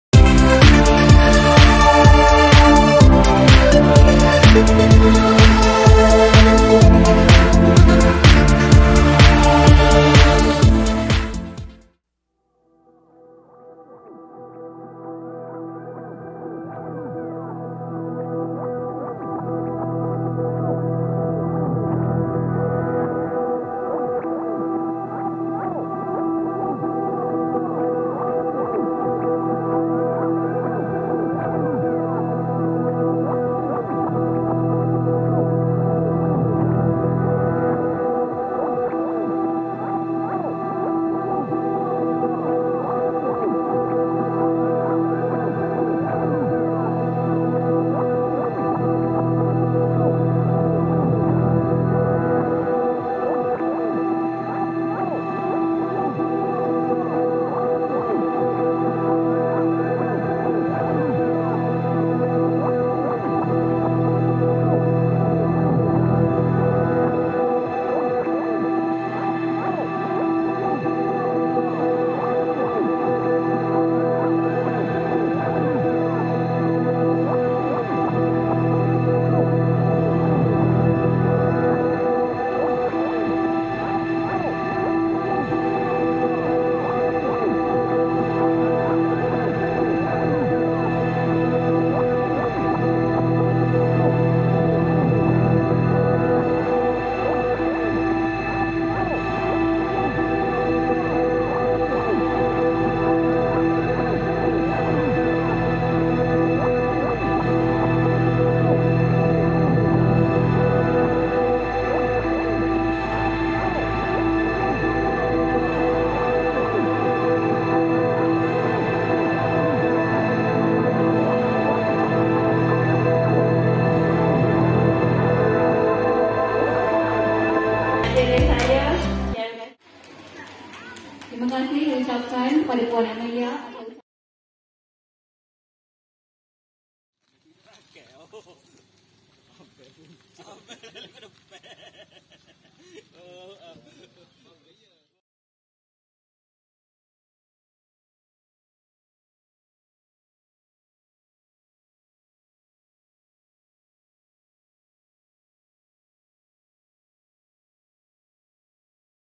Selamat Maju Jaya CIKGU AMEELIA di Tempat Baru
18 April 2022 - Kami Warga Pendidik Serta Kakitangan Sekolah Kebangsaan Pos Legap mengucapkan Tahniah dan Selamat Maju Jaya Kepada Puan Ameelia Nazirah, di atas pertukaran ke Sekolah Kebangsaan Raja Perempuan Muzwin, Kuala Kangsar. Terima Kasih Di Atas Jasa Serta Pengorbanan Yang Telah Dicurahkan. Semoga Terus Cemerlang Di Tempat Baharu.